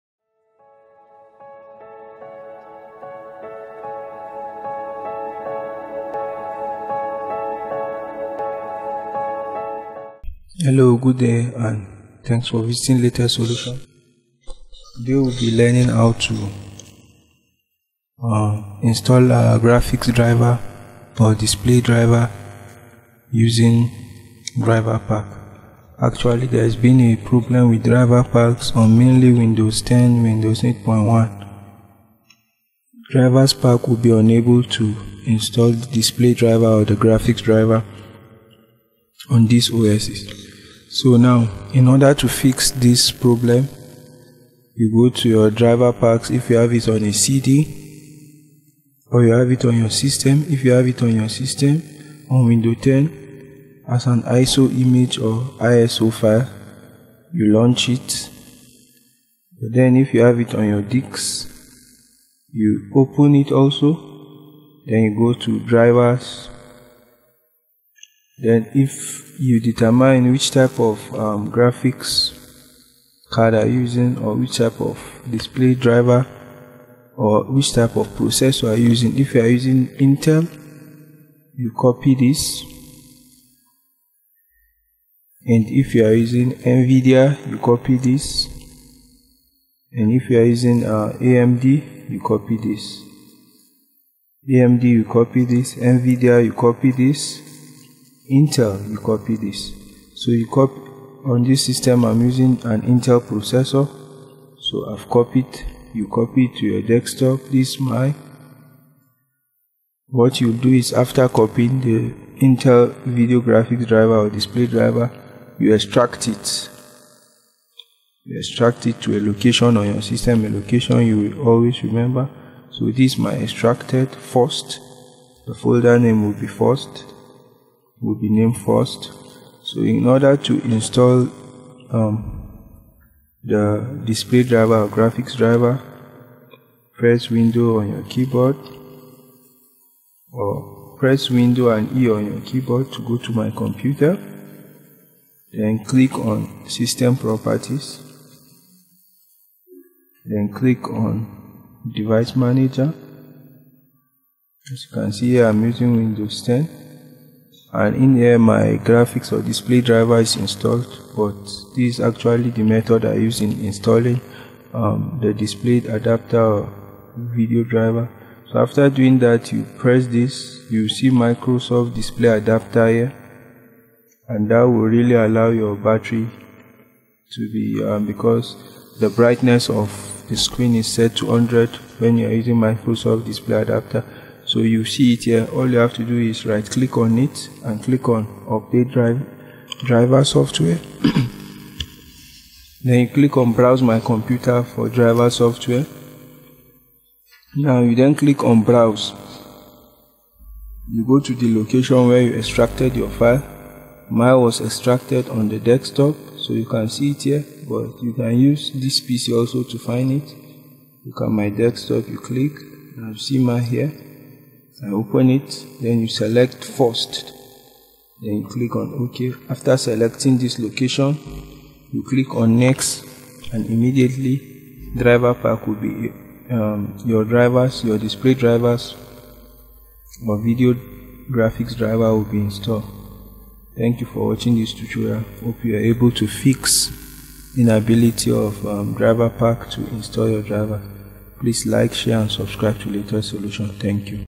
How to Fix DriverPack Solution not installing Graphics or Display Driver
Fix my battery running down quick or fastly on windows 10 with DriverPack.
Fix brightness level not changing on PC using DriverPack